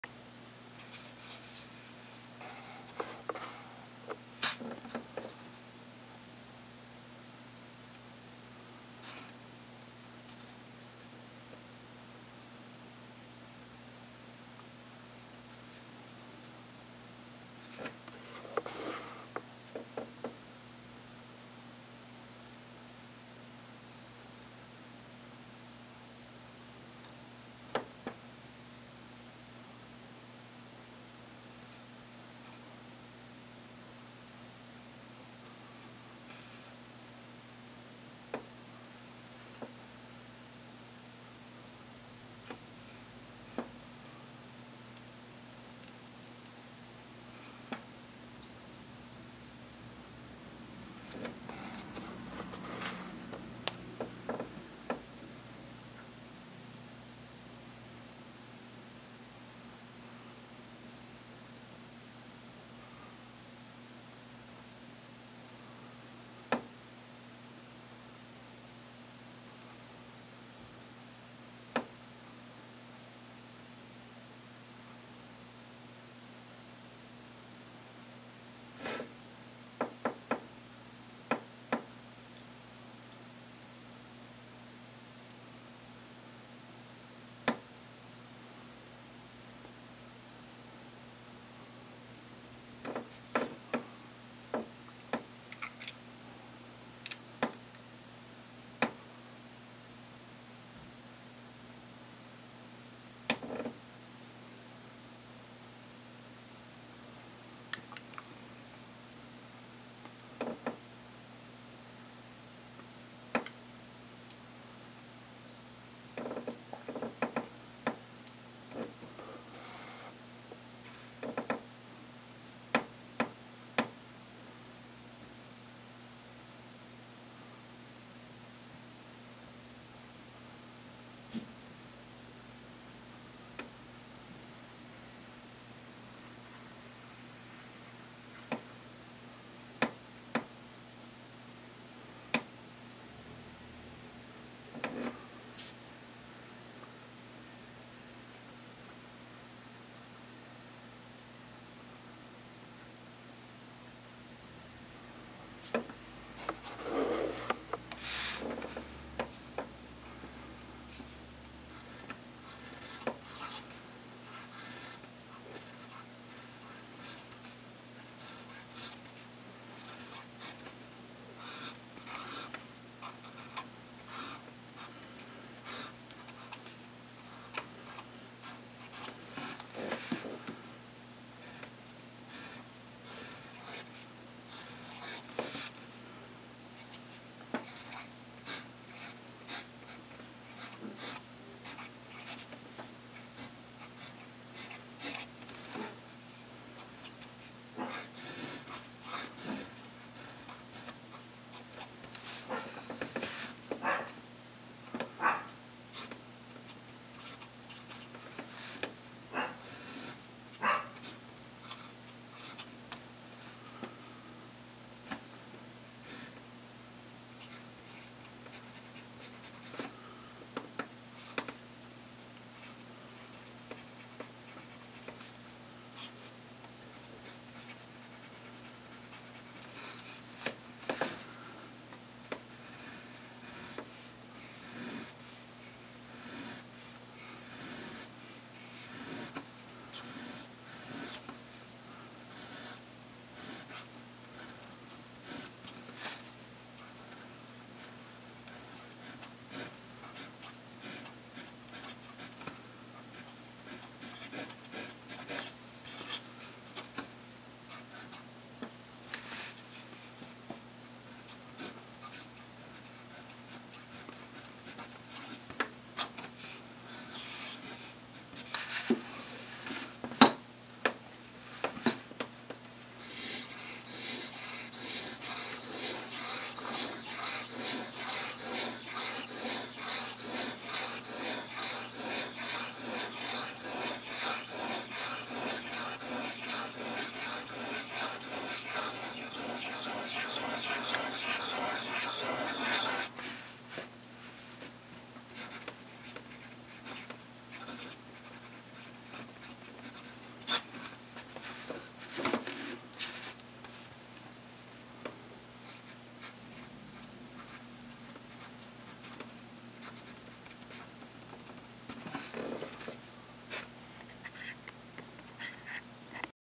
Vintage 1950's Pelikan 400 - Green Striated Celluloid - OBB Nib - Flex
this is a vintage Pelikan 400 in green striated celluloid fitted with an oblique double-broad nib.

Diamine Turquoise on Dot Rhodia